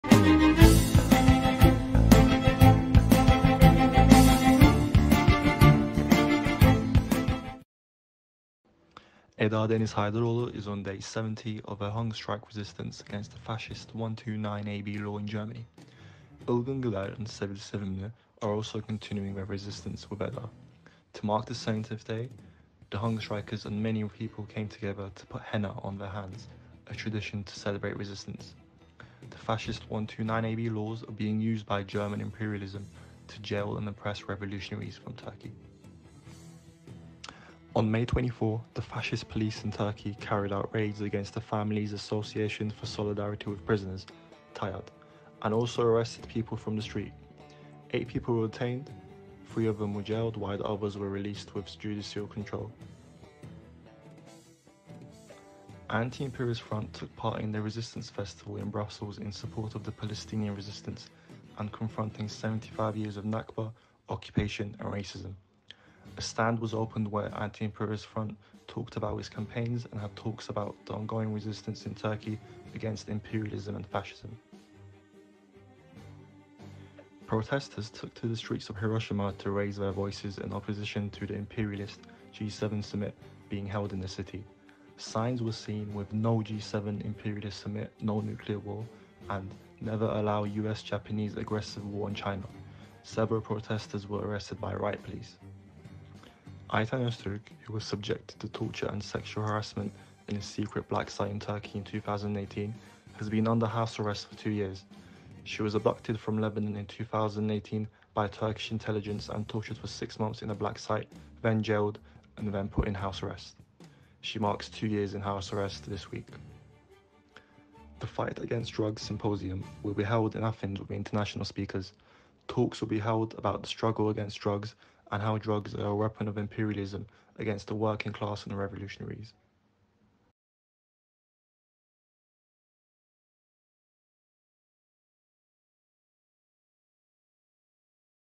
AIFnews | 28 May 2023 | Weekly News Digest
Here's your weekly news digest you won't hear in pro-imperialist media:

Eda on day 70 of hunger strike against fascist 129A/B law, Resistance Festival in Belgium for Palestine, G7 Protests in Japan, Police raids against People’s Front in Turkey...

Watch now!